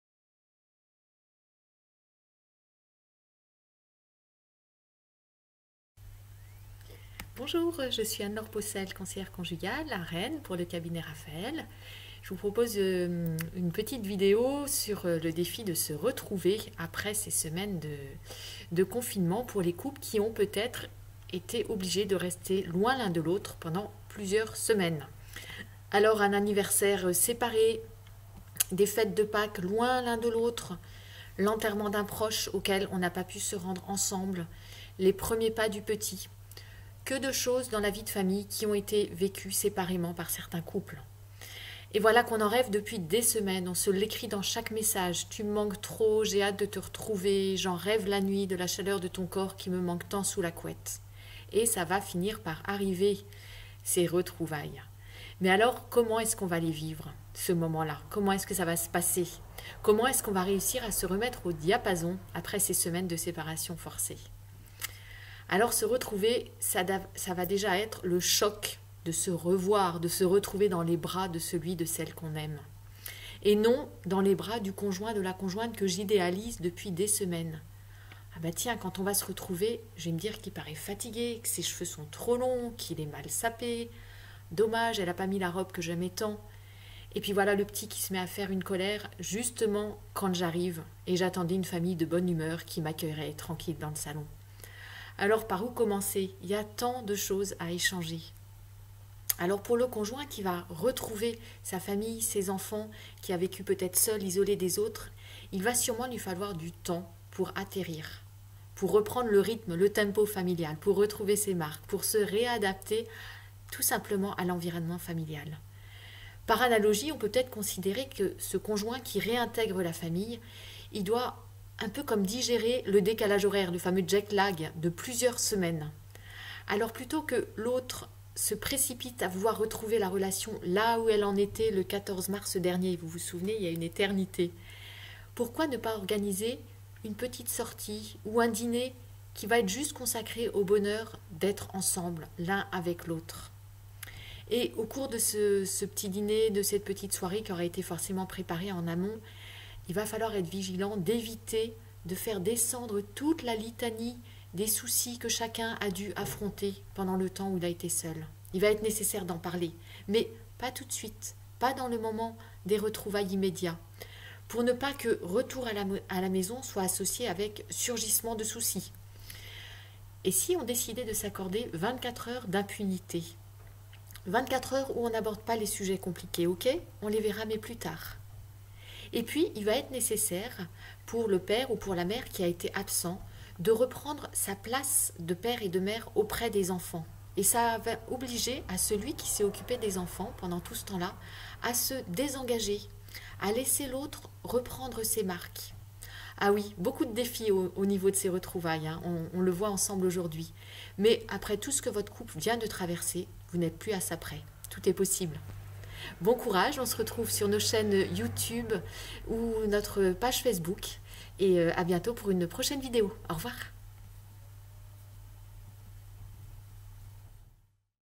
SE RETROUVER APRES LE CONFINEMENT - Petites astuces de Raphaël pendant le confinement
Couples séparés contre leur gré pendant le confinement, comment allez-vous vous retrouver le 11 mai ?
Quelques astuces pour préparer ces retrouvailles hors normes, proposés par Anne-Laure Peaucelle, conseillère conjugale et familiale au Cabinet Raphaël.